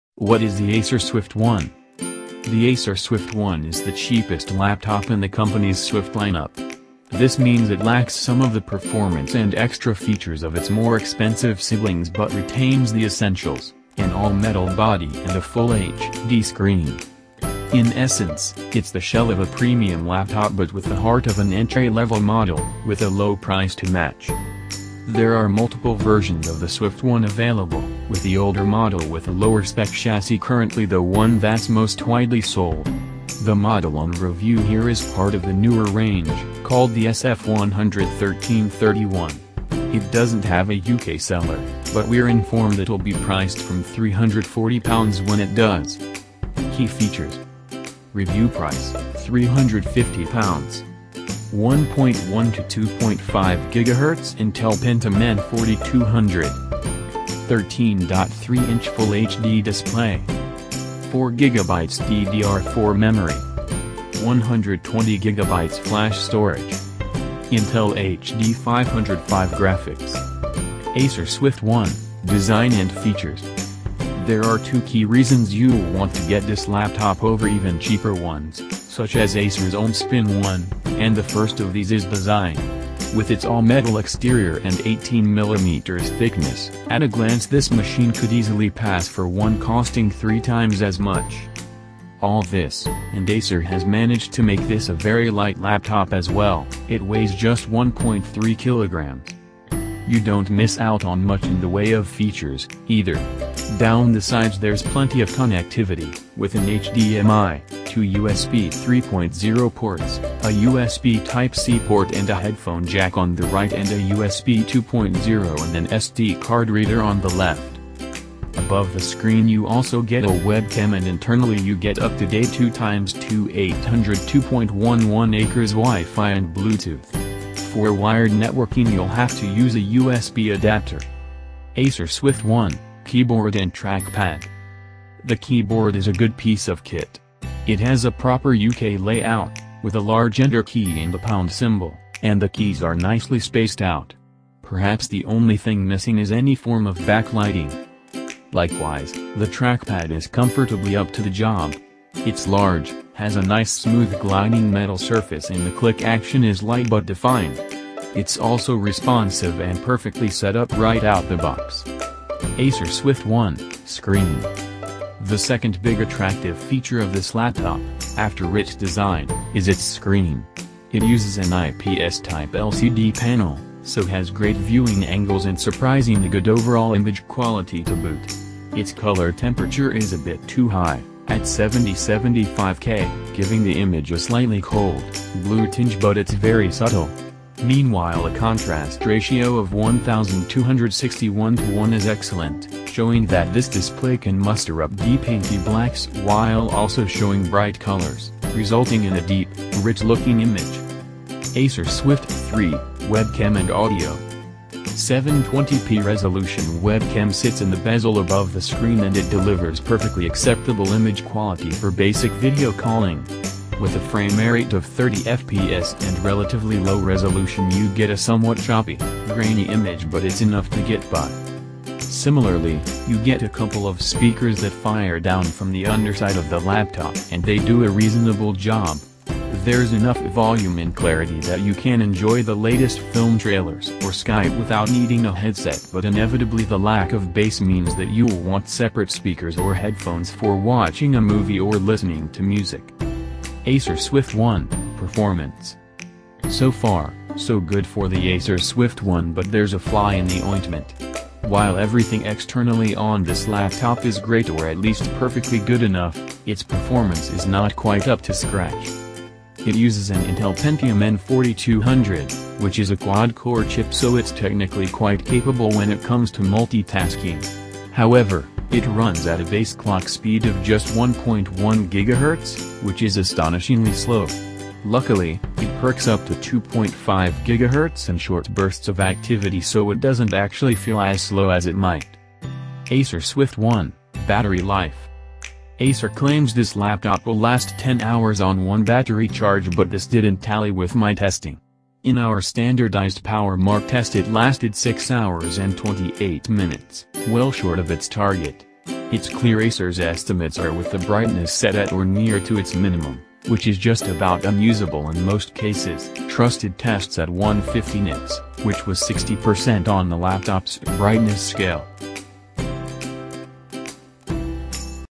HOT NEWS Acer Swift 1 review
This is a great laptop for anyone that tends to take their computing at a leisurely pace, but that doesn’t want to compromise on design and screen quality. If you mainly use a laptop for typing, watching video and checking email, it’s ideal.

If you’re more of a power user that likes to have half a dozen browser tabs open at once, while flicking back and forth to your email, having Spotify running in the background and just generally being a little busier, then the sluggishness of the Swift 1 will prove a

frustration. You’ll have to bite the bullet and spend a little more for a machine running a Core i3 processor and an SSD.